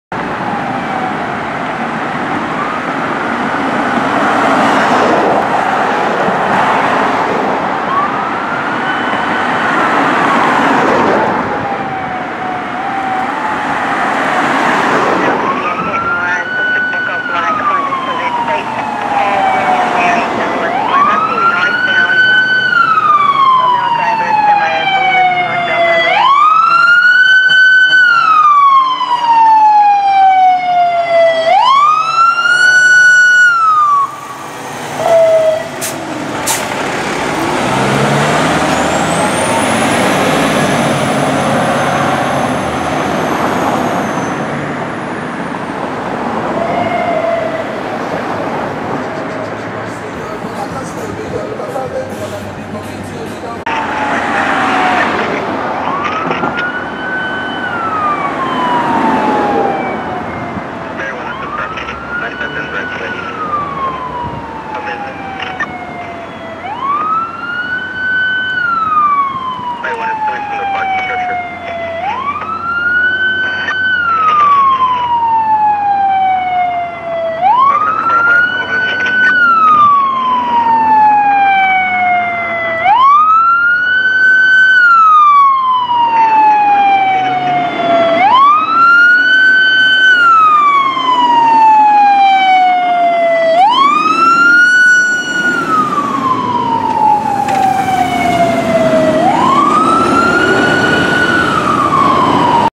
Beverly Hills Fire Dept. Engine 1 Responding
Clip One: Beverly Hills Fire Department Engine 1 responding and arriving on scene of an automatic alarm at the 400 block of north Oakhurst Drive.

Units Due: E1

Clip Two: Beverly Hills Fire Department Engine 1 responding along with a BHPD motor unit to a man down in a vehicle at the 200 block of north Oakhurst Drive. The subject was only sleeping and didn't need medical attention.

Units Due: E1 RA3

Beverly Hills, CA - 10/19 & 10/21/17